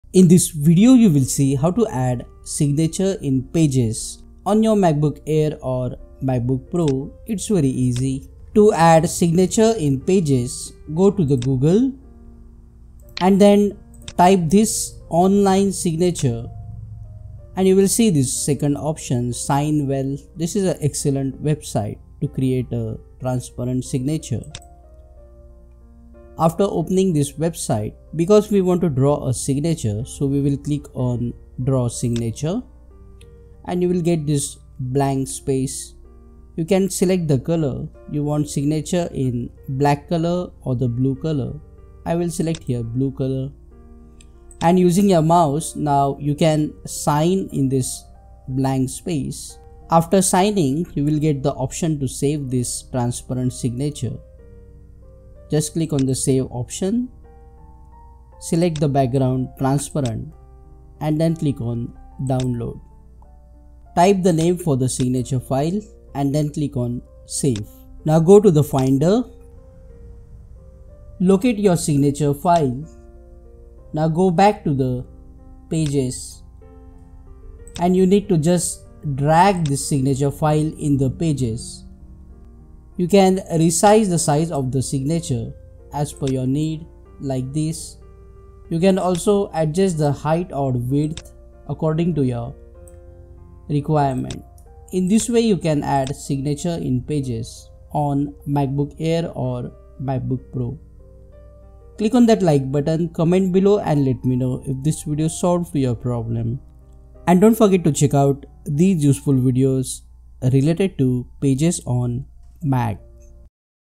How to add Signature in Pages [ MacBook ]
In this Apple MAC Pages Tutorial Video Learn How to add Signature in Pages on MacBook Air or Macbook Pro. The inserted signature will have a transparent background.